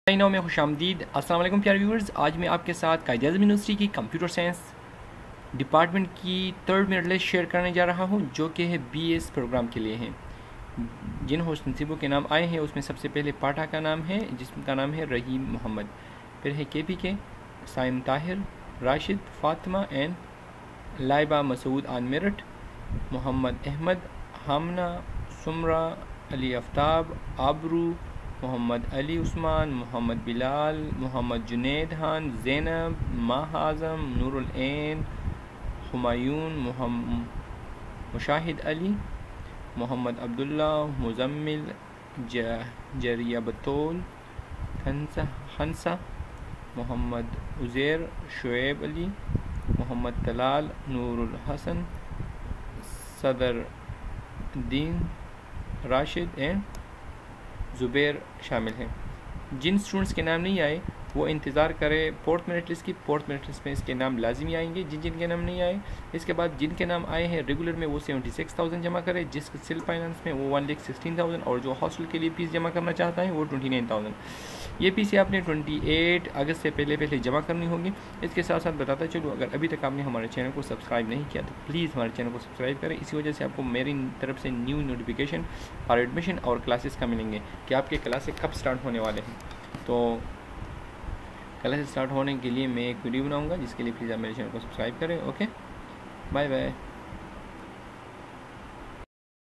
Computer Science QAU Third Merit List for Undergraduate Programme (Fall-2020)
Third Merit List for Undergraduate Programme (Fall-2020)
Computer Science
FEE/DUES DETAILS FOR FIRST SEMESTER
   S.# DESCRIPTION OF FEE/DUES RATES PER SEMESTER
   1 REGULAR Rs. 76,030/- (Approximately)
   2 SELF FINANCE Rs. 116,336 /- (Approximately)
   3 HOSTEL Rs. 29,500/- (Approximately)
Please download the fee challan form from your Admission Portal Account Dashboard and pay in your nearest HBL/Askari Bank till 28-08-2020. After Depositing fee, please Upload the Paid Challan from your Admission Portal Account Dashboard to confirm your admission till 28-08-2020.

In case of incorrect/misleading information or concealment of facts, the University reserves the right to refuse/cancel the admission and expel the applicant (student) from the University at any time.

Please verify your merit before depositing fee. In case of any wrong information regarding domicile and marks will automatically lead to cancellation of admission

NOTE 1:
The priorities/preferences would remain intact till the applicant gets admission against his/her first priority/preference OR finalization of the admission process, whichever is earlier. The dues/fee paid by the applicant according to the earlier merit list would automatically be shifted/adjusted if his/her name appeared in the subsequent list (s) against the filled priorities/preferences.

Search:
# ID NAME FATHER QUOTA AGGREGATE
1 UF20-04010 RAHIM MUHAMMAD SARDAR,ALI FATA 83.5818
2 UF20-07254 SAIM TAHIR JAVED,TAHIR KPK 87.0182
3 UF20-13065 RASHID . ZAMAN,GUL KPK 87.0000
4 UF20-01827 FATIMA TUZ ZUHRA KHAN,MUHAMMAD MAJID SAEED KPK 86.9576
5 UF20-08153 LAIBA MASOOD MASOOD,MUBARAK MERT 91.4147
6 UF20-10320 MUHAMMAD AHMAD ALAM,MAHMOOD PNJB 90.4469
7 UF20-06441 HAMNA NADEEM NADEEM,SHEIKH MUHAMMAD PNJB 90.4126
8 UF20-07098 SUMRA HABIB REHMAN,RAJA HABIB UR PNJB 90.4100
9 UF20-09872 ALI AFTAB BHATTI,MUHAMMAD AFTAB PNJB 90.2973
10 UF20-17616 AABROO FATIMA SHAH,SYED JAVVAD HAIDER PNJB 90.2929
11 UF20-15266 MUHAMMAD ALI USMAN YAQOOB,MUHAMMAD PNJB 90.1979
12 UF20-11611 MUHAMMAD BILAL SALAMAT ALI,SALAMAT PNJB 90.1434
13 UF20-00868 MUHAMMAD JUNAID KHAN ALTAF,MUHAMMAD PNJB 90.0976
14 UF20-13102 ZAINAB ARSHAD ARSHAD,MUHAMMAD PNJB 90.0517
15 UF20-17118 MAHA AZAM MUGHAL,MUHAMMAD AZAM SELF 86.2703
16 UF20-09517 NOOR UL AIN SHOAIB,MUHAMMAD SELF 86.2430
17 UF20-01266 HUMAYUN IHSAN HAQ,IHSAN UL SELF 86.2016
18 UF20-04143 MUSHAHID ALI WASSAN,SANAT ALI SELF 86.1626
19 UF20-03022 MUHAMMAD ABDULLAH AKRAM,MUHAMMAD SELF 86.1219
20 UF20-00376 MUZAMMIL BEHROZ KHAN,BEHROZ SELF 85.8364
21 UF20-15677 JARIA BATOOL HAIDER,SAJJAD SELF 85.8084
22 UF20-00941 KHANSA SAFDAR ABBASI ABBASI,SAFDAR HUSSAIN SELF 85.6923
23 UF20-00182 MUHAMMAD UZAIR KABEER KABEER,MUHAMMAD SELF 85.6650
24 UF20-16014 SHOAIB ALI MUHAMMAD,SHER SELF 85.6128
25 UF20-00252 MUHAMMAD TALAL PERVAIZ IQBAL,PERVAIZ SELF 85.5427
26 UF20-11364 NOOR UL HASSAN MUMTAZ,FAIZ SELF 85.5276
27 UF20-14288 SADAR DIN DIN,ALIF SNDR 87.0235
28 UF20-16015 RASHID ALI ABBASI,AHMED ALI SNDR 87.0235
29 UF20-18155 ZUBAIR ZAHID